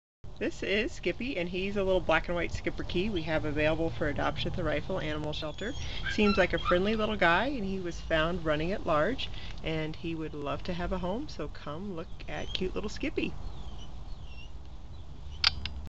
Skipper 3 year old Schipperke Rifle Animal Shelter
Skipper is a very friendly little 3yr. old or so Schipperke mix. He's all black with a little white on his chest and chin. His coat has a wave to it. He's a very well behaved and well mannered dog and is a real find! Don't wait if you are looking for an awesome little dog! He's fun to spend time with and would be great for about anyone. We haven't had him with other dogs, but he is great with them on the fence line. He was found as a lost dog and has been a super dog with us. He is a little camera shy and his video does not show his maximum cuteness. Come and meet this cutie!

The adoption fee for dogs is $50.  All our dogs are spayed/neutered and up to date on their vaccines.  Please leave a message if no one answers.  We are often busy and can't get to the phone.  Thank you for thinking adoption first!